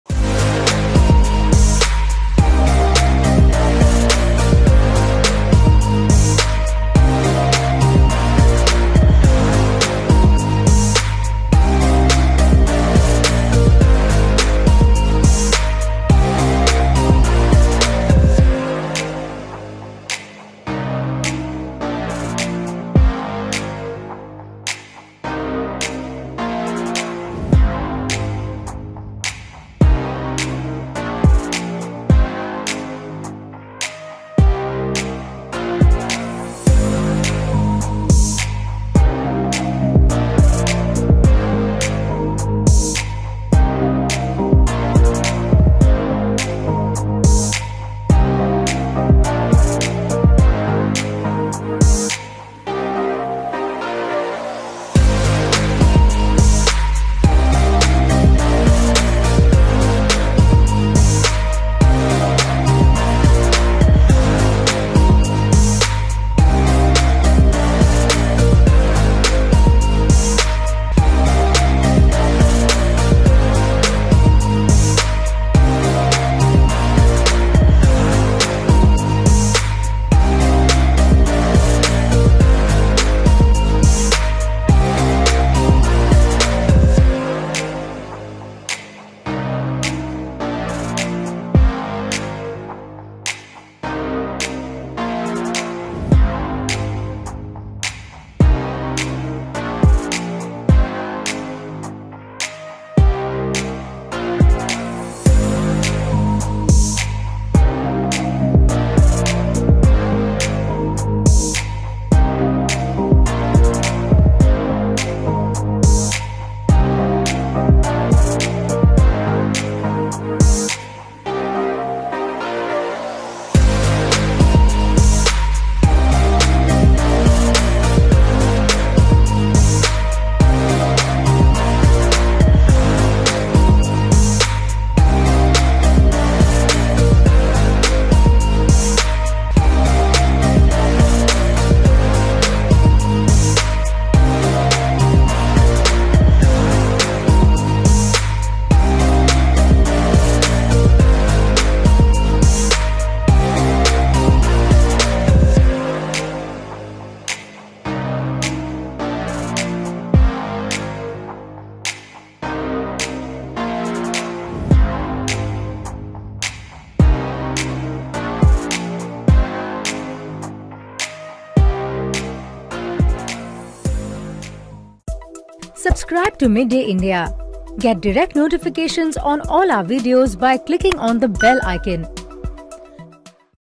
Bigg Boss 15: Fans disappointed over Vishal Kotian and Neha Bhasin’s shocking eviction
New twists and turns peak in the famous TV show BiggBoss15. NehaBhasin, JayBhanushali , and VishalKotian were evicted on Thursday. However, the eviction did not go well with Kotian and Bhasin’s fan. The fans flooded social media asking the makers to bring them back.